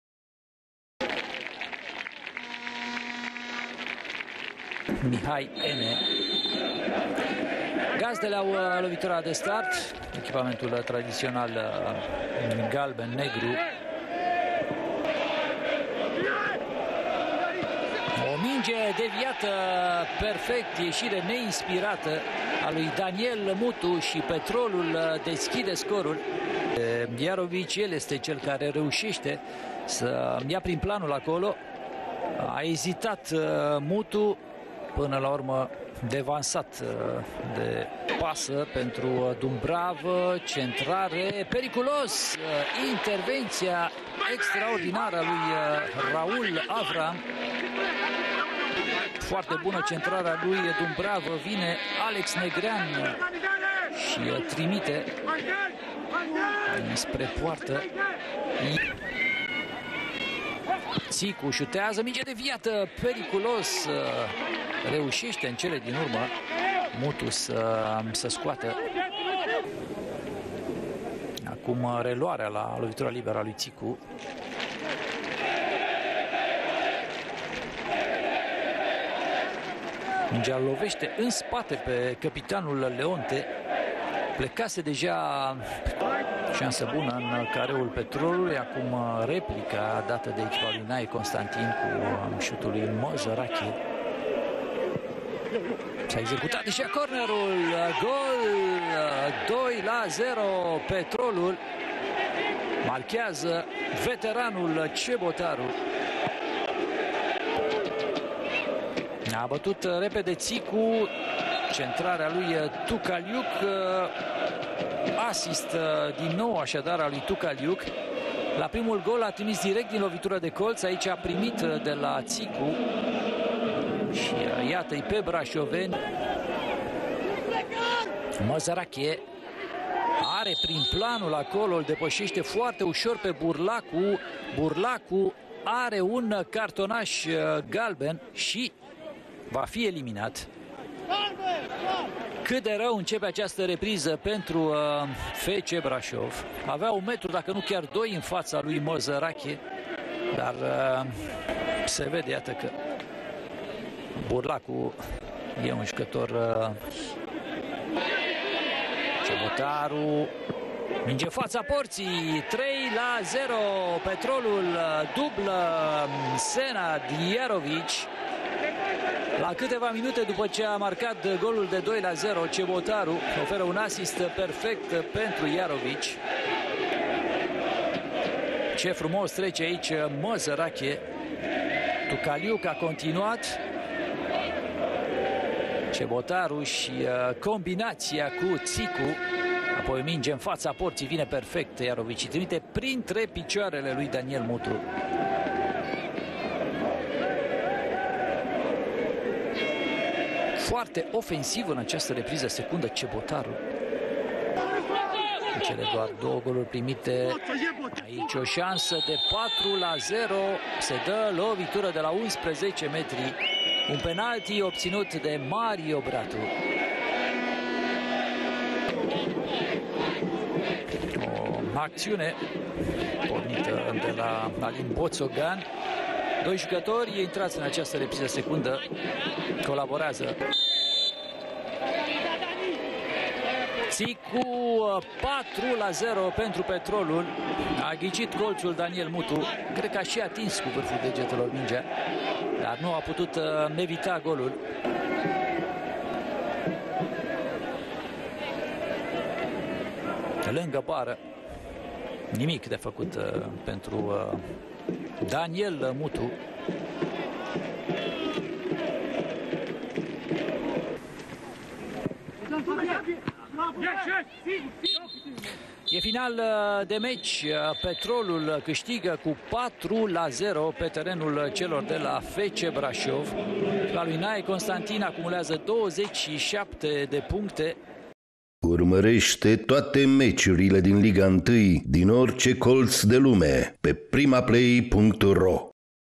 REZUMAT Liga 2: FC Braşov - Petrolul 0-4
Petrolul a făcut scor la Braşov şi îşi consolidează primul loc în clasamentul ligii secunde. Jarovic, cu o dublă, Cebotaru şi Ţicu au marcat pentru "lupii galbeni".